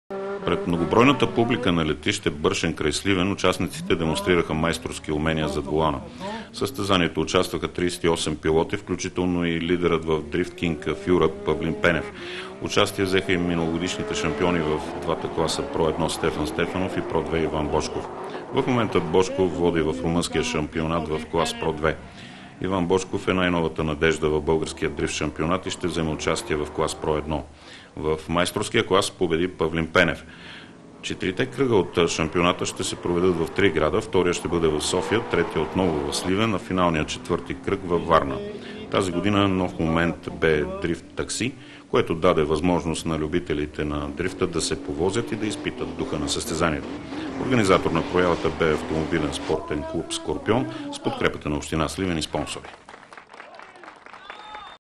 Приключи първи кръг от българския дрифт шампионат
Първи кръг от Българския дрифт шампионат в Сливен приключи. Той се проведе през почивните дни на пистата на летище „Бършен“